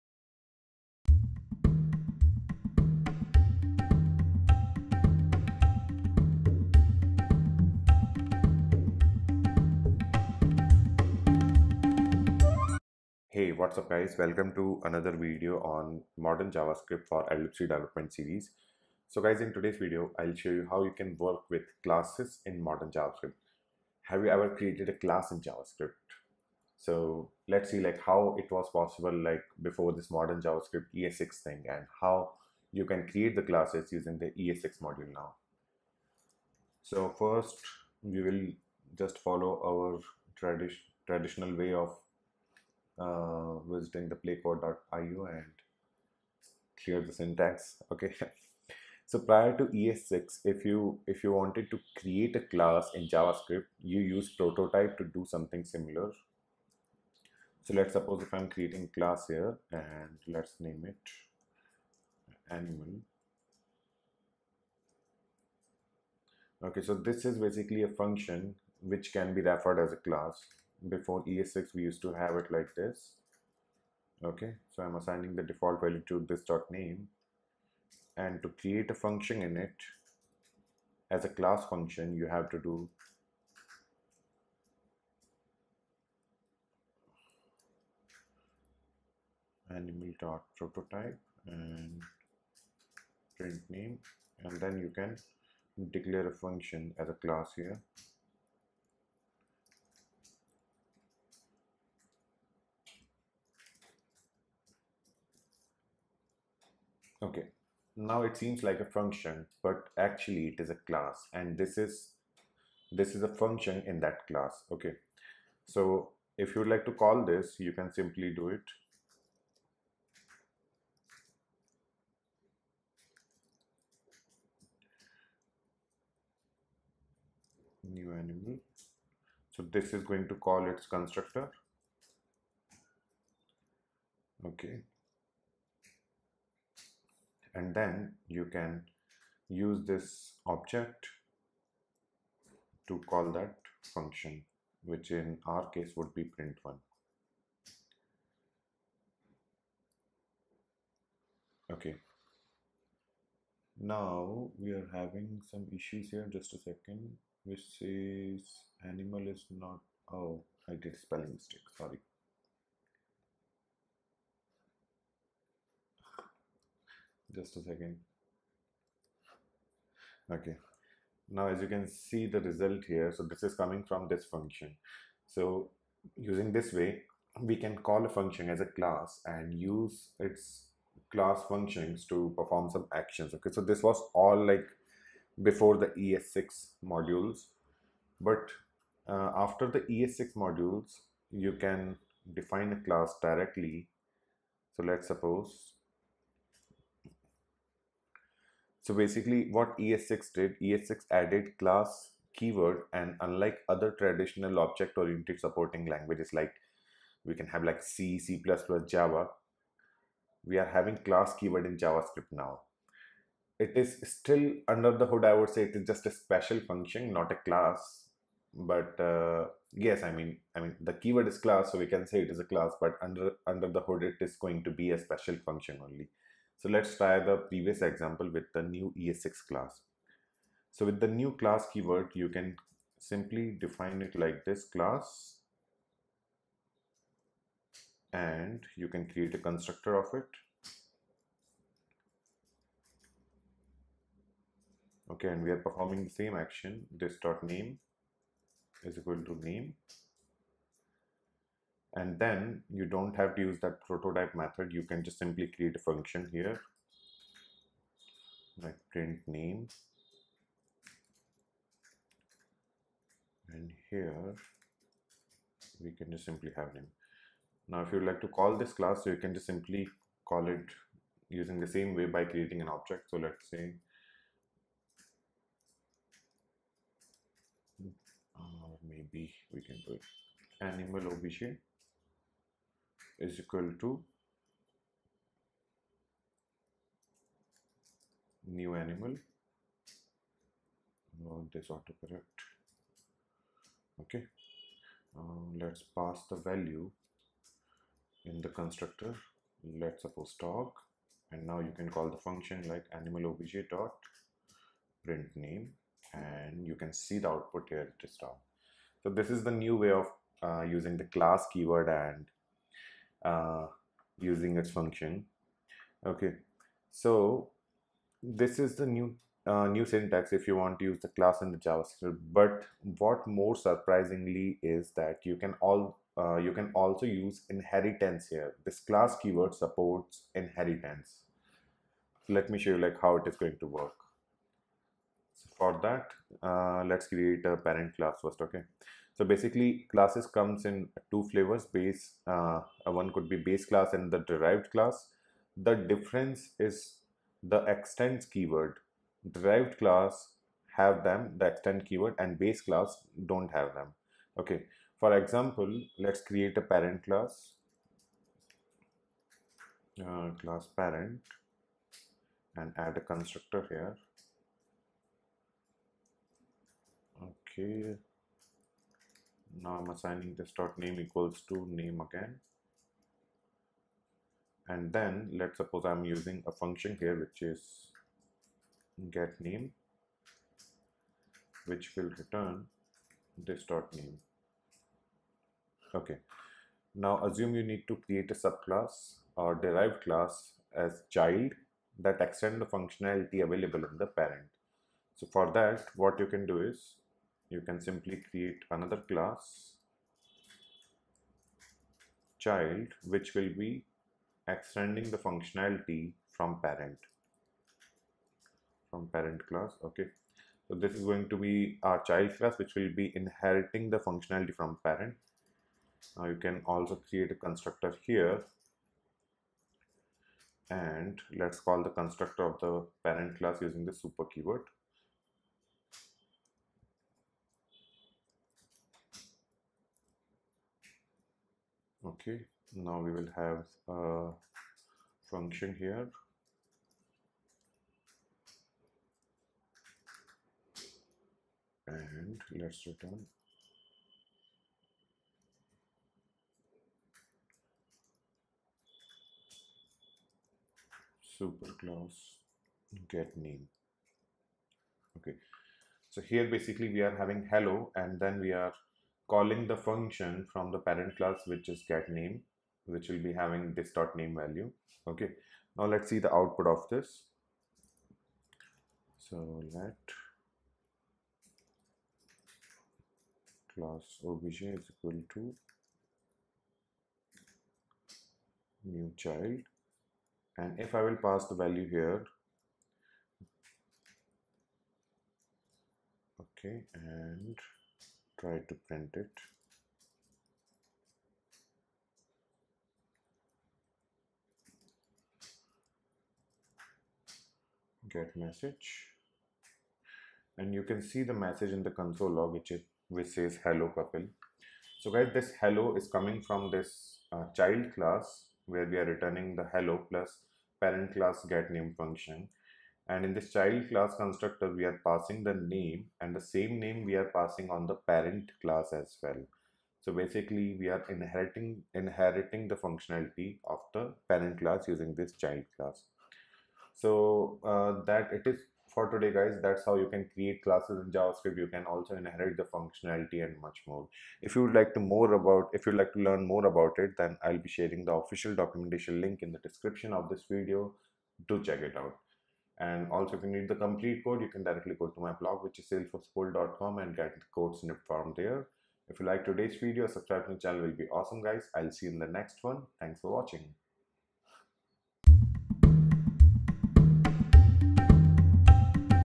#6 Work with Classes in Modern JavaScript | LWC
ES6 added class keyword and unlike other traditional object-oriented supporting languages like java or c++ ,class keyword in Javascript. Still under the hood it’s just a special function.